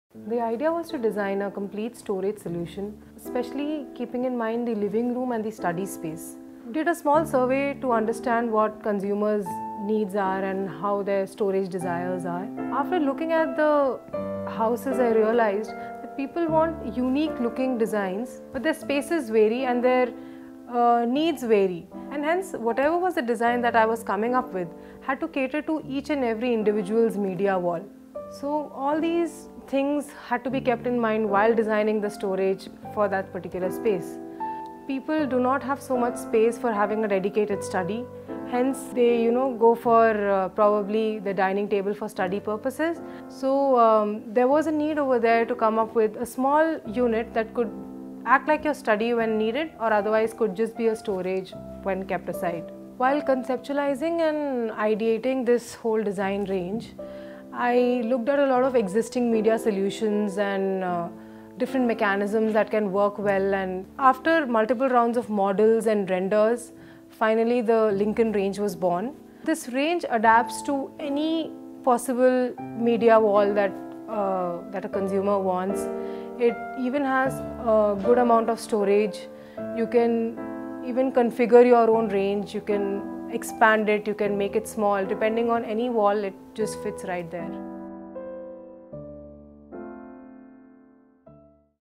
TV Units & Living Storage Design: Lincoln Living Room Essentials
High definition. Defined by smooth angular lines and a clear, simple frame, the Lincoln TV unit is contemporary chic. Cleverly designed with a mix of open and closed storage, there’s plenty of room for your media players, sound system, books, movie collections, and more. The tall cabinets on either side are perfect for speakers, while the open back allows for better wire management. The raised base allows for easy cleaning below the unit.